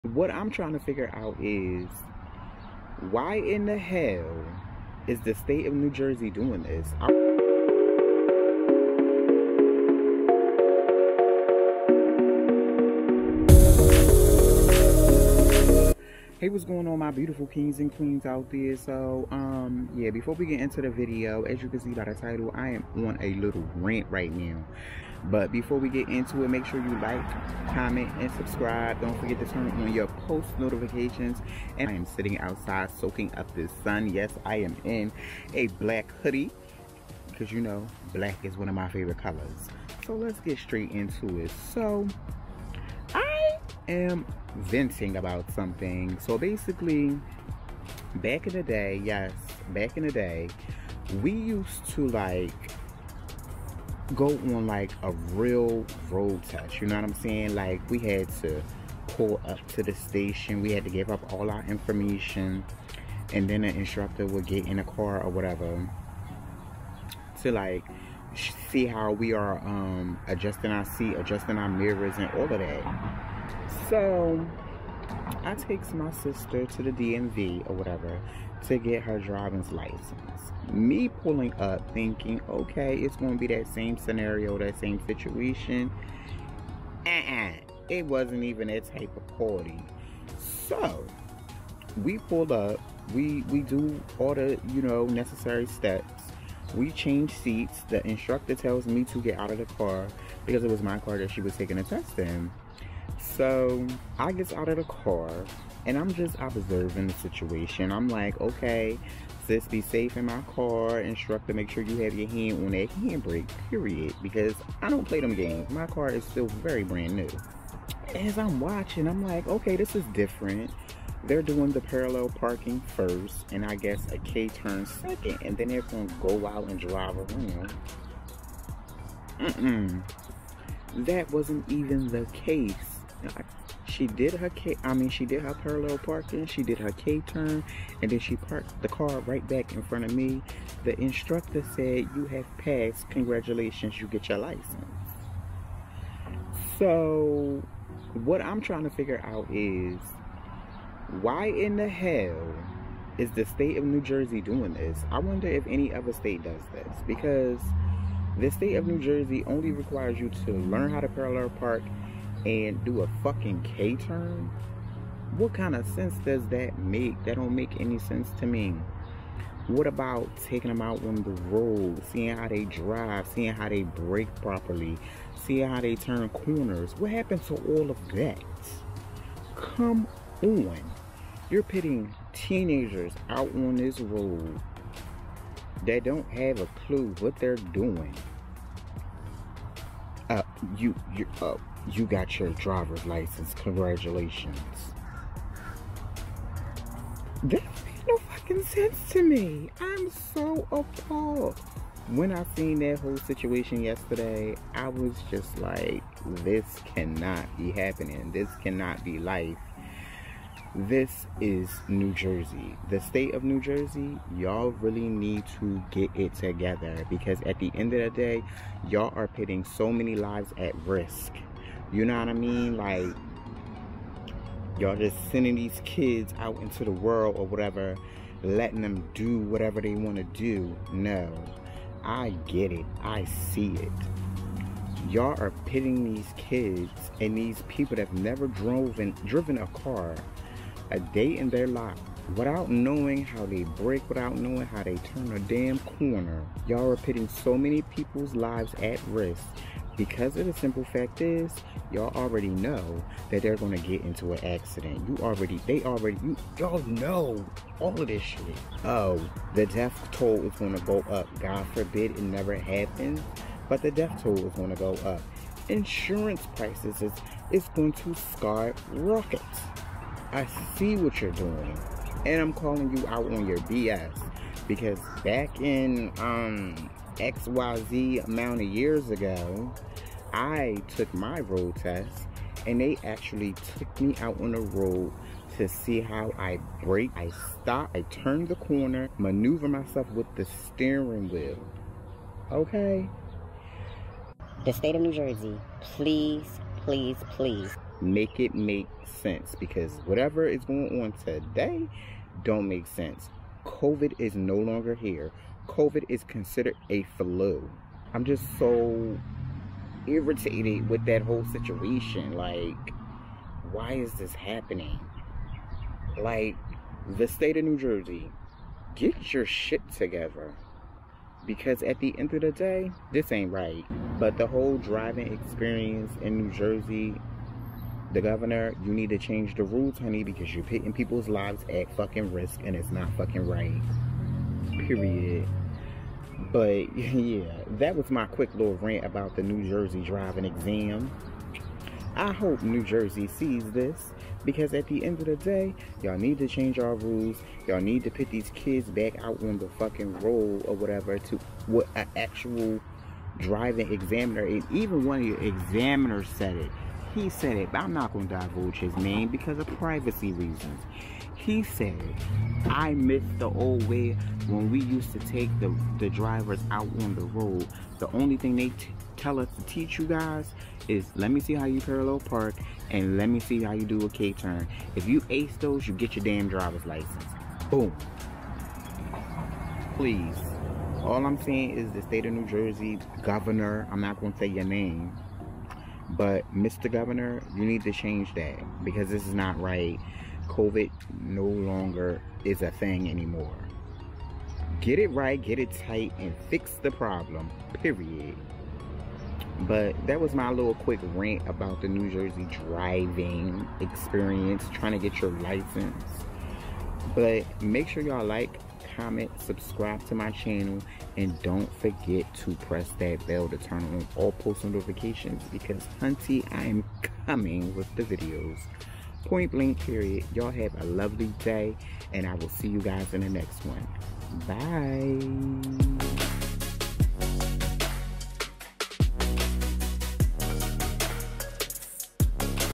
NJ DRIVING EXAM RANT!!!
This driving exam is so messed up on so many different levels it’s not even funny.